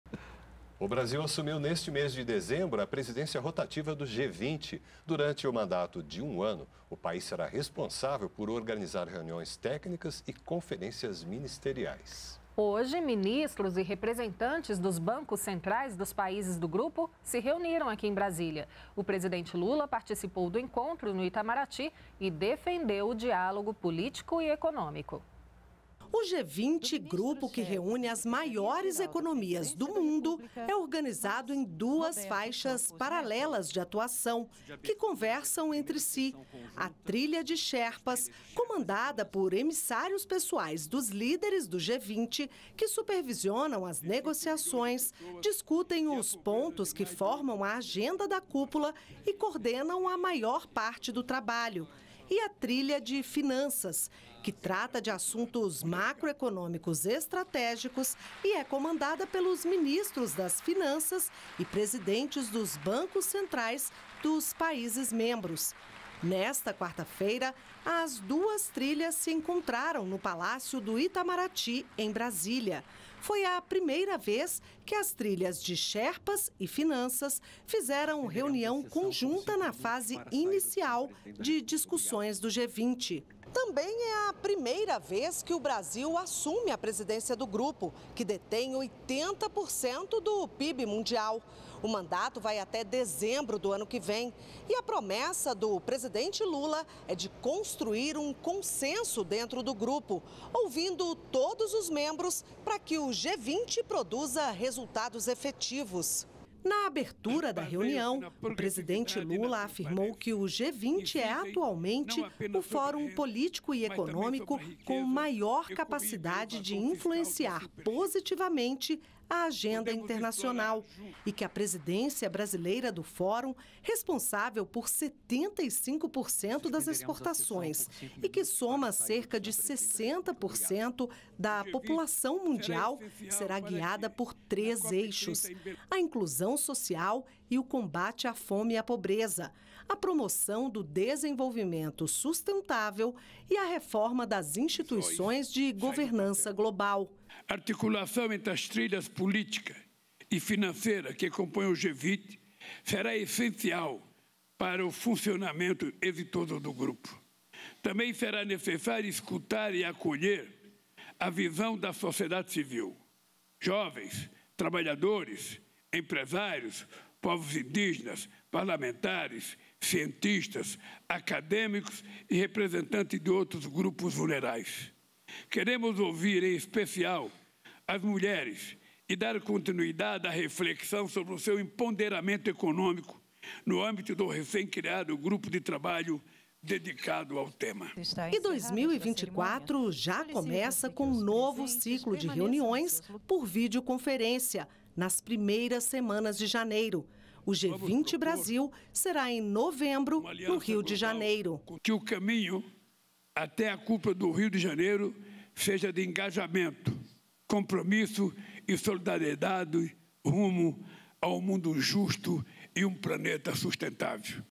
MINISTROS E REPRESENTANTES DE BANCOS CENTRAIS DO G20 SE REÚNEM EM BRASÍLIA
Com o Brasil na presidência rotativa do bloco, ministros e representantes de bancos centrais do G20 se reuniram em Brasília. O presidente Lula participou do encontro e defendeu o diálogo político e econômico.